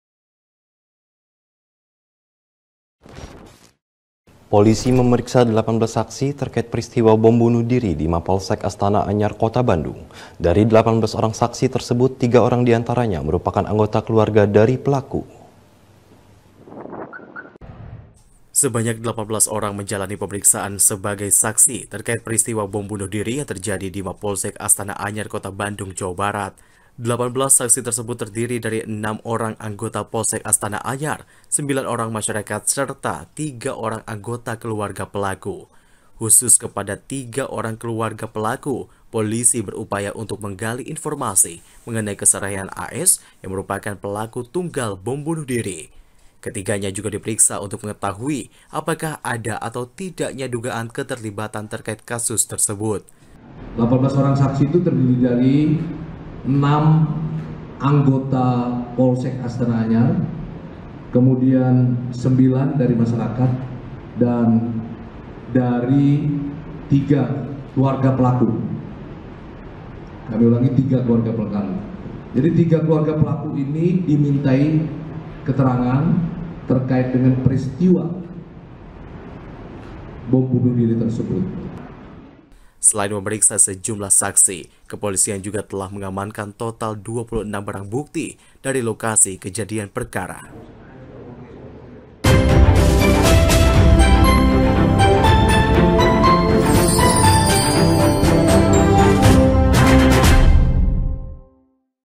Polisi Periksa Keluarga Pelaku Bom Bunuh Diri
Polisi memeriksa 18 saksi terkait peristiwa bom bunuh diri di Mapolsek Astana Anyar Kota Bandung. Dari 18 orang saksi tersebut, tiga orang diantaranya merupakan anggota keluarga dari pelaku.